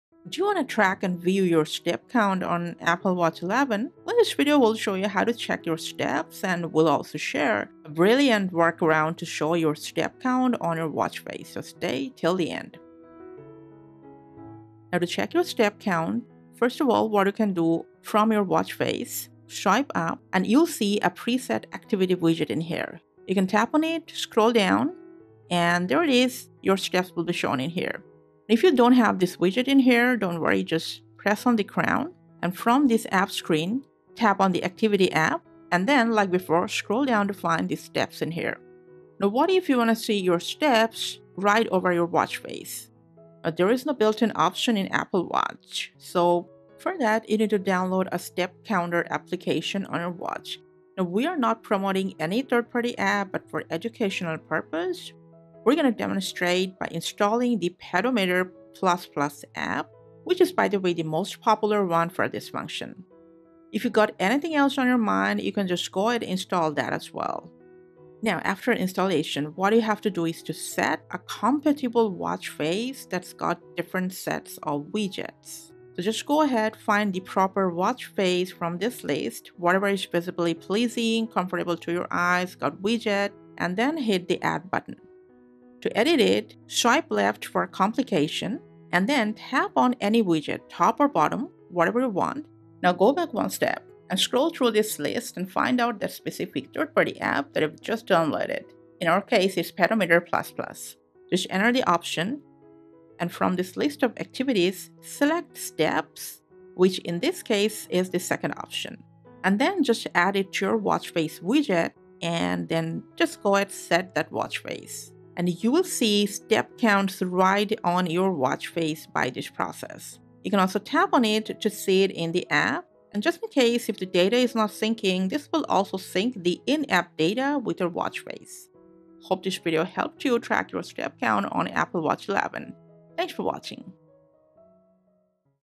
How To Track Steps With Apple Watch 11! [Watch Face]
Can't track your step count with Apple Watch Series 11, so want to view how many steps you've walked today? Need help to show your daily step count on the Watch Face of your Apple Watch 11 for easy viewing? No worries,  you've come to the right video.

Let's check out the step-by-step beginner's guide to see your daily step count on Apple Watch 11 from the Activity widget or App effortlessly. We'll also show you how to add and display daily step count on the Apple Watch 11 Watch Face as well.

0:00 Intro: Track Steps With Apple Watch 11
0:14 Check Step Count on Activity Widget & App
0:41 Show Step Count on Watch Face